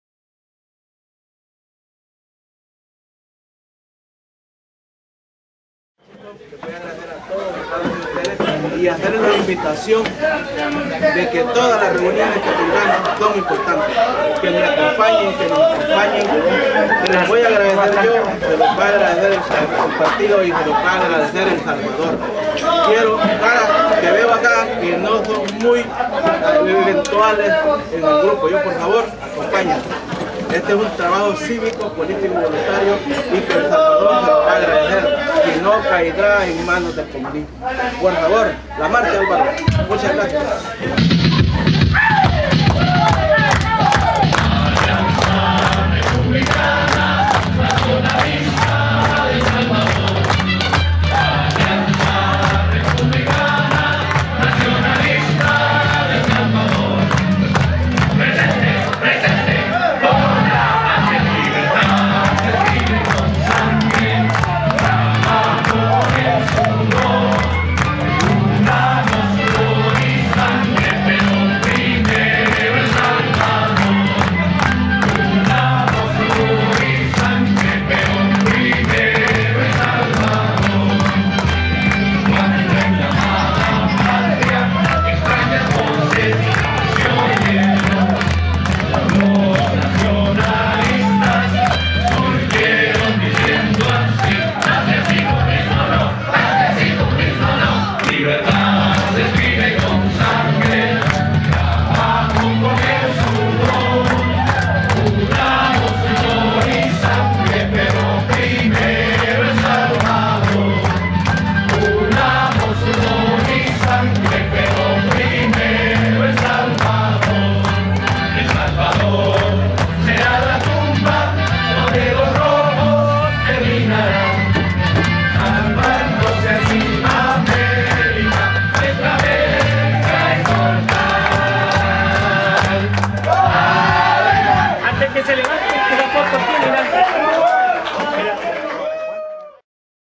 Concluye reunion con Roberto D'Aubuisson
El diputado Roberto D'Aubuisson concluye su reunion con simpatizantes y miembros del Sector 8 de Virginia.